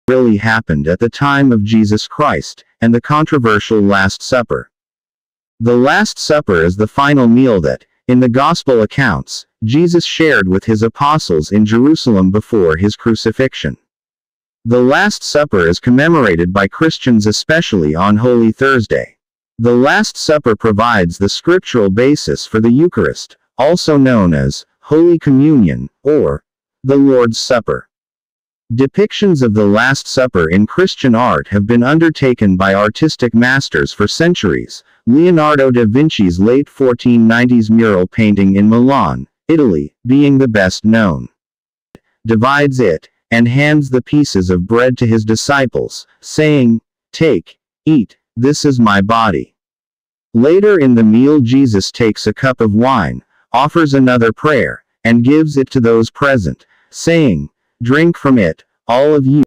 Trivia: Ano Ang Nangyari Pagkatapos Ng Huling Hapunan
The Last Supper is the final meal that, in the Gospel accounts, Jesus shared with his apostles in Jerusalem before his crucifixion. The Last Supper is commemorated by Christians especially on Holy Thursday. The Last Supper provides the scriptural basis for the Eucharist, also known as "Holy Communion" or "The Lord's Supper".